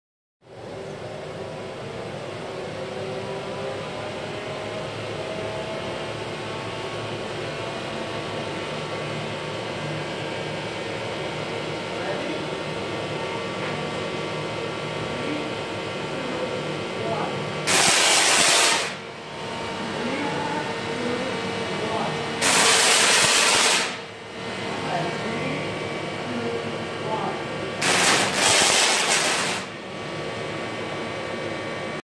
Hava da elektrik iletir. Ve Ampul yanar
Tesla Muzesi Belgrad Sirbistan , Binlerce volt elektrik hava ile iletilebilir ama carpilmazsiniz